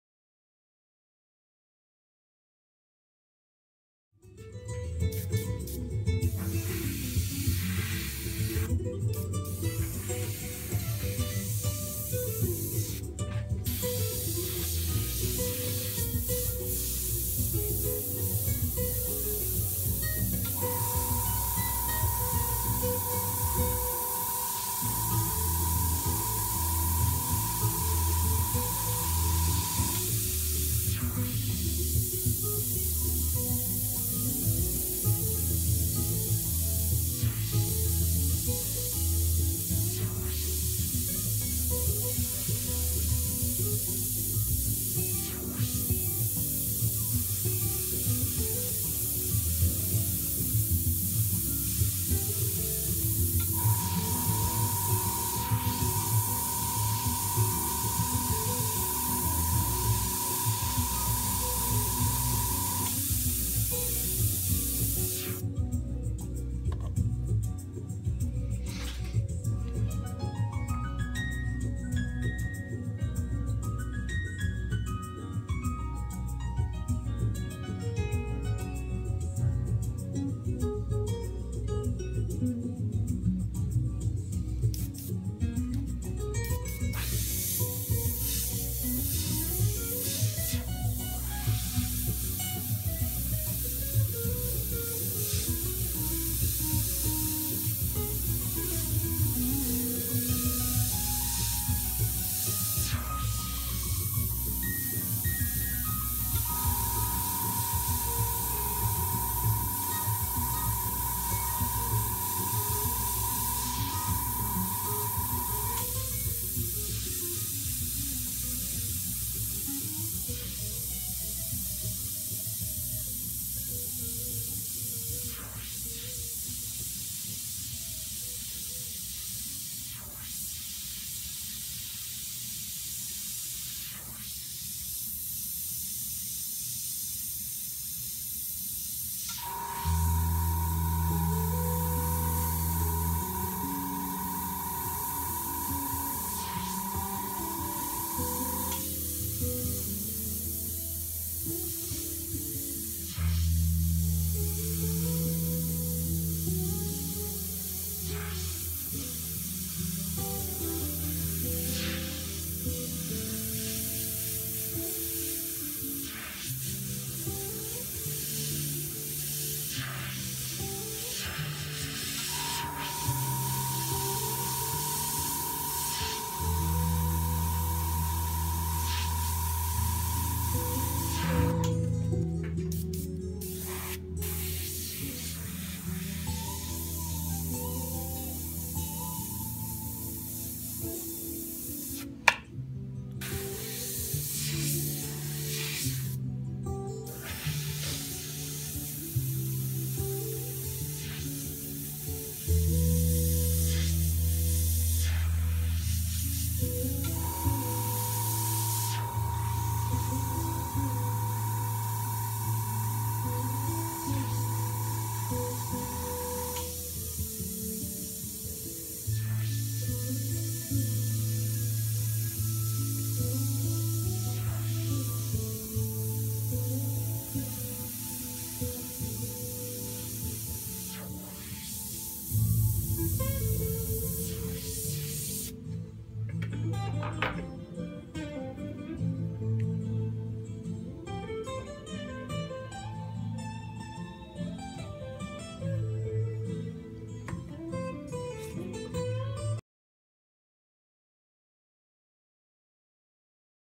Model horse airbrushing dapples
Painting dapples on a buckskin model horse using an Iwata micron gravity feed CM-B.

Buy your airbrush, paints and supplies here!

🐎🐎🐎🐎🐎🐎Follow me🐎🐎🐎🐎🐎🐎